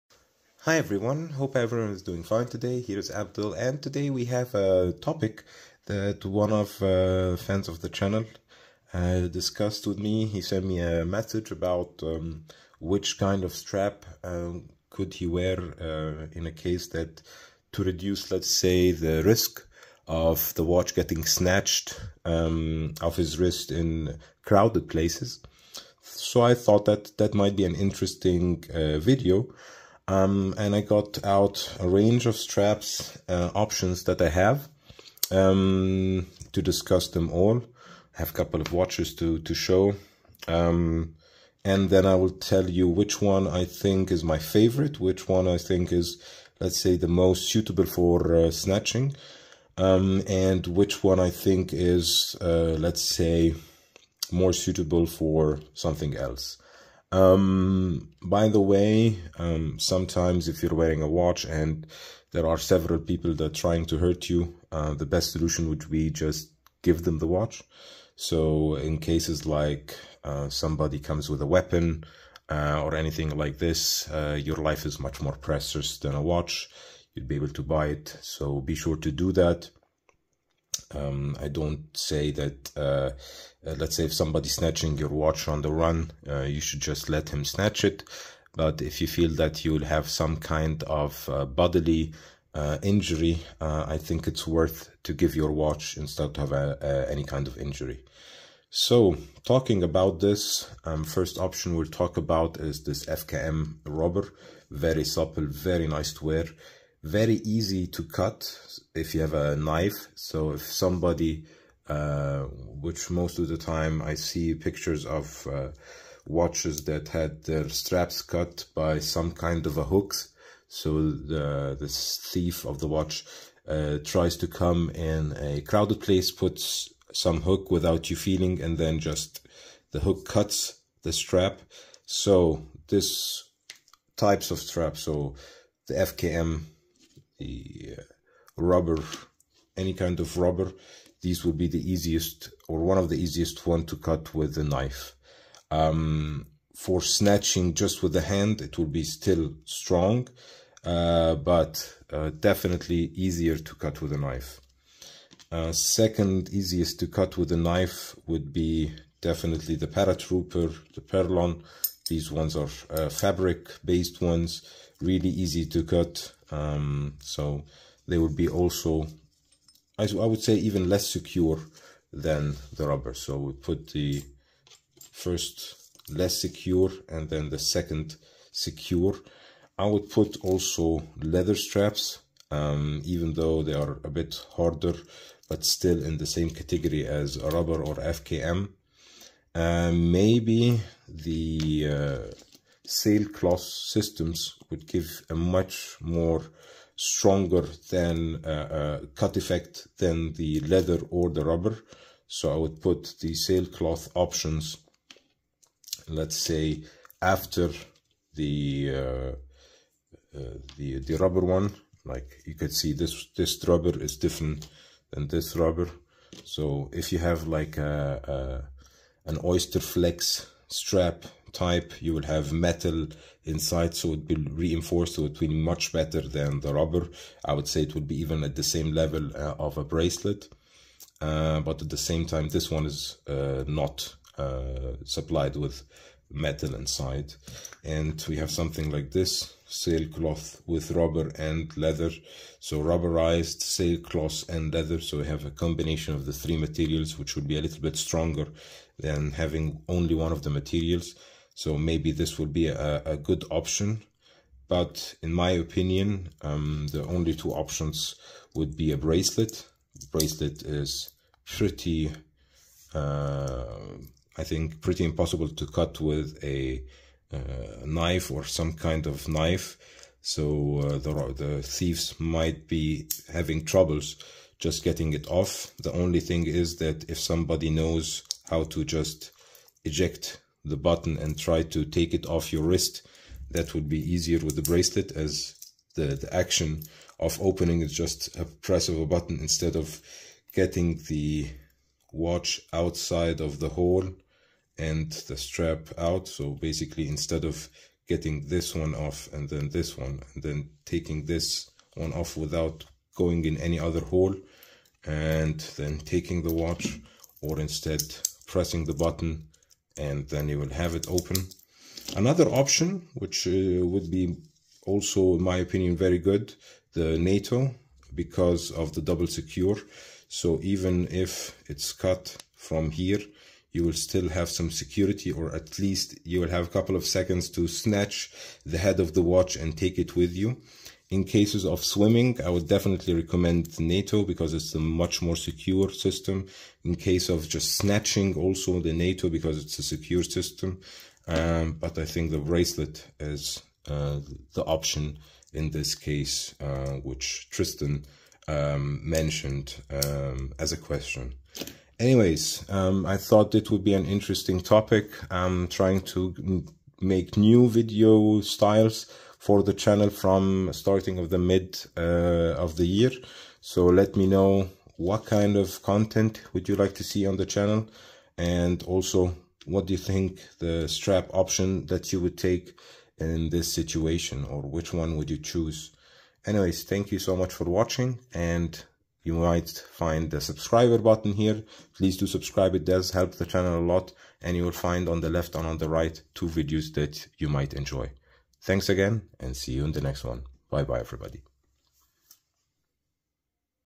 Guide To The Safest Watch Straps: Which One Should You Wear?
Welcome back to Abdul R Watches! Today, we're shifting our focus from the watch itself to an equally important aspect - the watch strap.

Safety is a key factor to consider when choosing a watch strap. Whether you're a sports enthusiast, a professional working in a demanding environment, or someone with sensitive skin, the right strap can make all the difference. In this video, we explore a variety of watch straps, delving into their safety features, comfort levels, and durability.

We will discuss materials from leather to metal, fabric to rubber, and everything in between. We'll consider factors such as hypoallergenic properties, resistance to sweat and water, and the potential for causing skin irritation or discomfort.

Remember, the strap is not just a style statement; it's an extension of you and your lifestyle. Watch this video to learn how to make the safest and most comfortable choice.

Your feedback helps us create content that serves you better.

Stay safe and keep exploring!